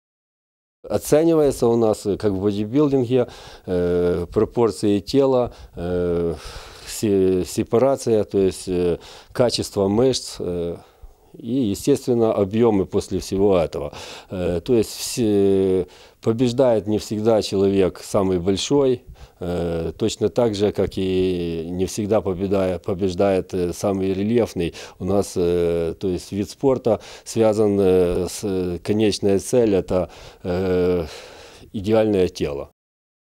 У Хмельницькому відбудеться битва тіл (2)
У Хмельницькому влаштують битву тіл. 7 травня наш обласний центр прийме другий відкритий Кубок України з бодібілдингу та фітнесу. Організовує його Хмельницька федерація силових видів спорту. На участь у конкурсі заявили  більше, як  півсотні учасників. Переможців обиратимуть в тридцяти категоріях.